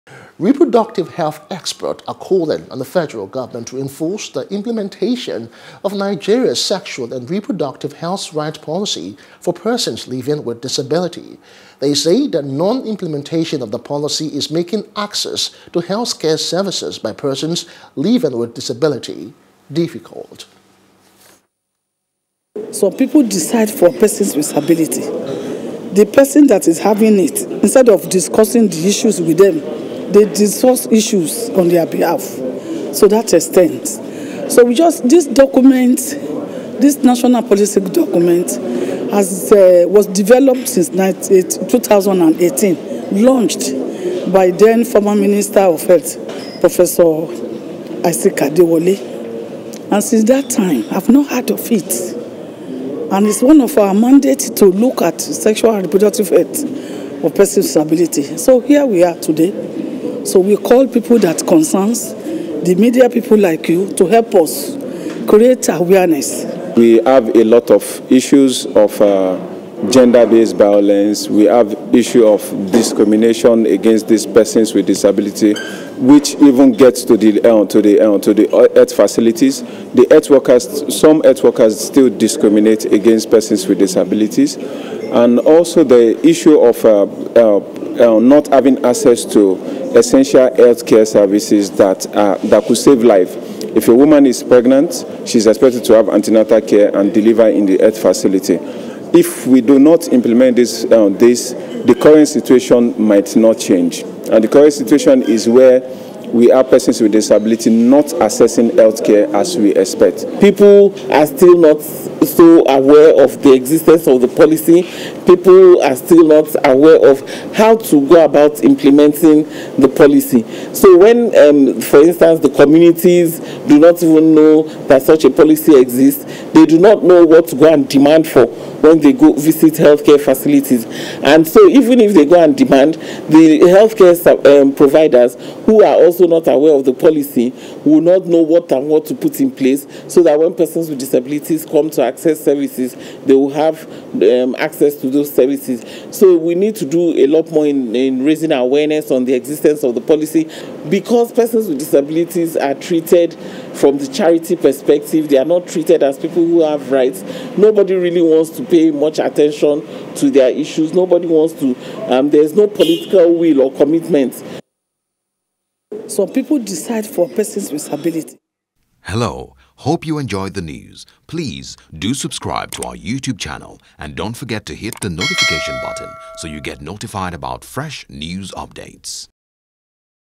Disability Health Rights: Experts Urge Implementation Of Health Policies | NEWS
Reproductive health experts are calling on the federal government to enforce the implementation of Nigeria's sexual and reproductive health rights policy for persons living with disability.